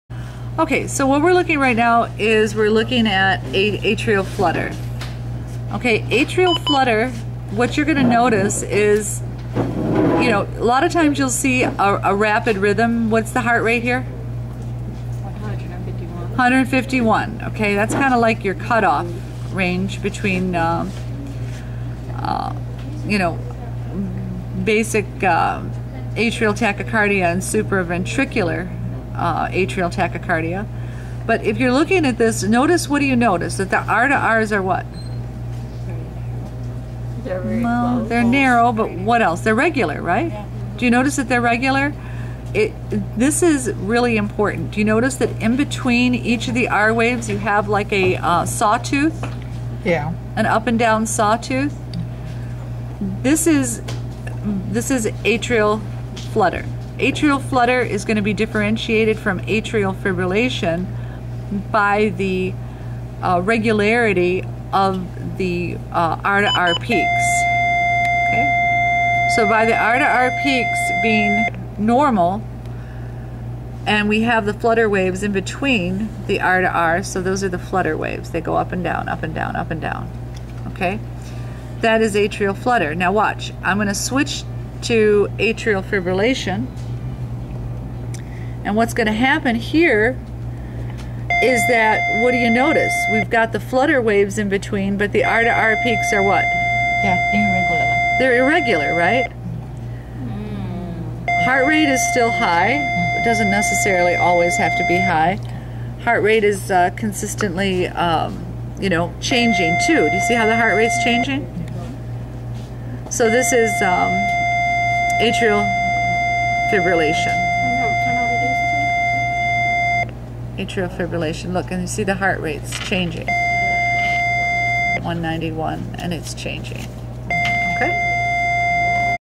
Telemetry Interpretation 4
Class is learning how to read and interpret cardiac arrhythmias